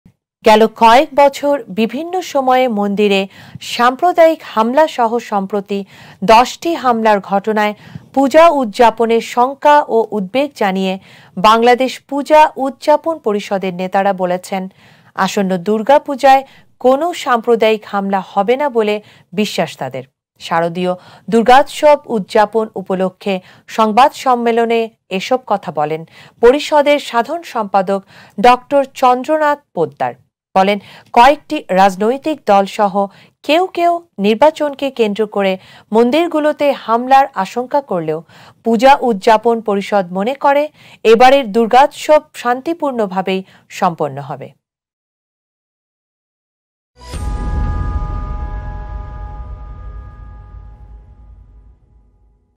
পূজা উদযাপন নিয়ে কেন শ’ঙ্কা’য় হিন্দুরা I Hindu I Puja I Hindu I Celebrating I
পূজা উদযাপন নিয়ে কেন শ’ঙ্কা’য় হিন্দুরা

Why are Hindus worried about celebrating Puja?